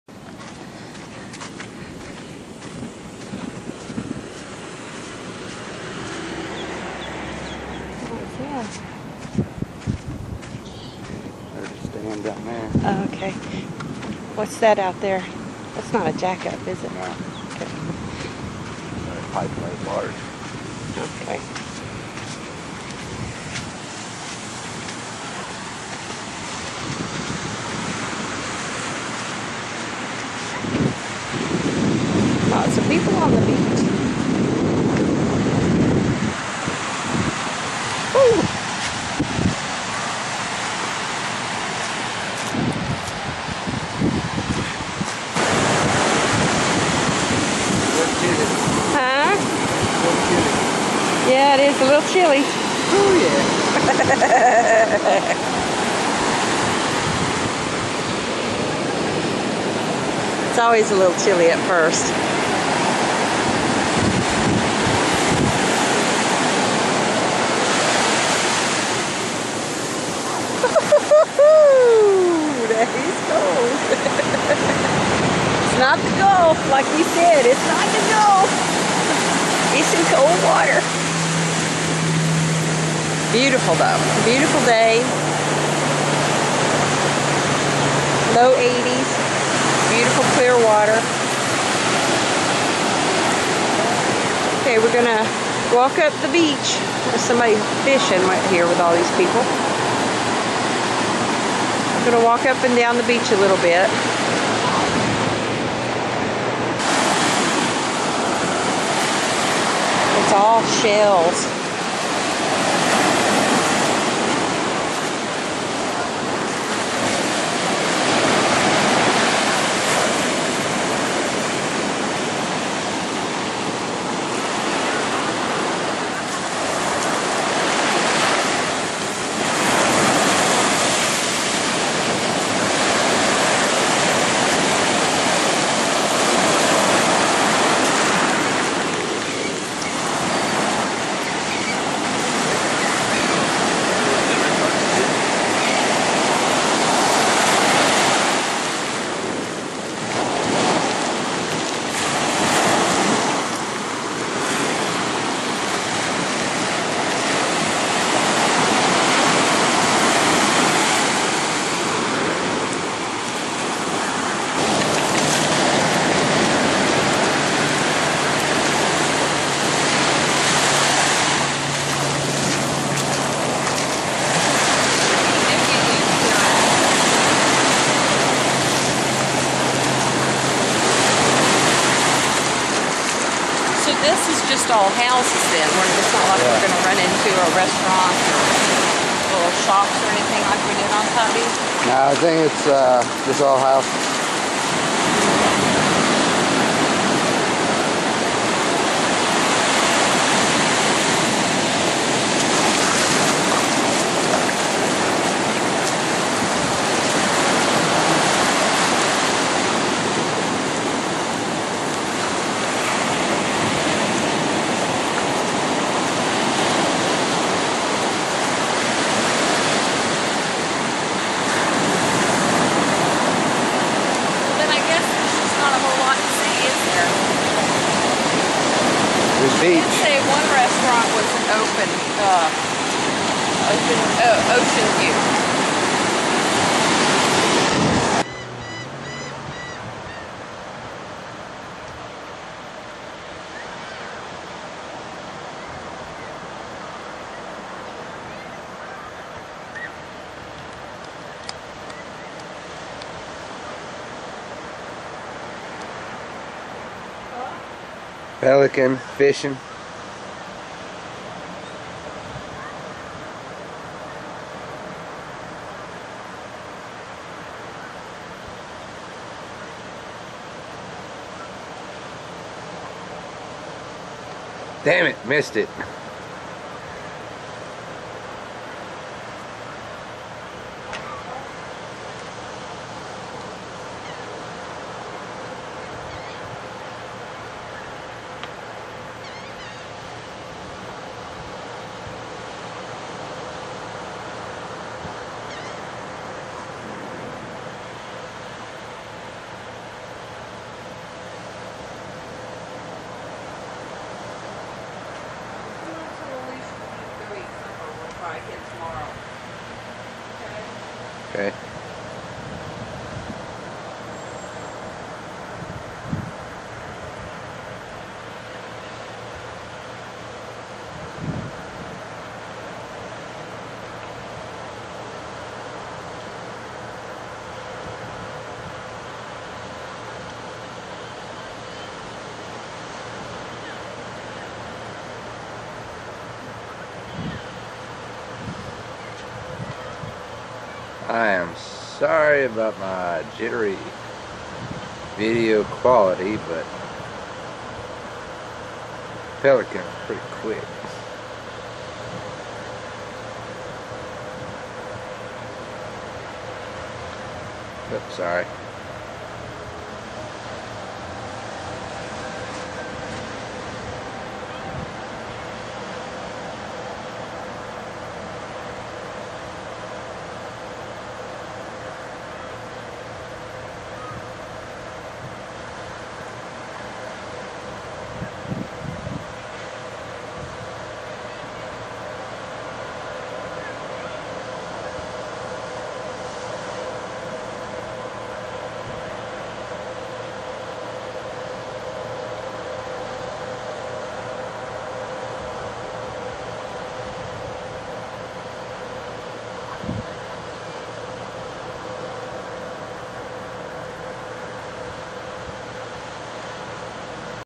Vilano Beach Fla April 10, 2012
Our first venture out to the beach on the first day there.  The Pelican was filmed from our balcony that evening.  I hope you enjoy watching.

Hugs
cheri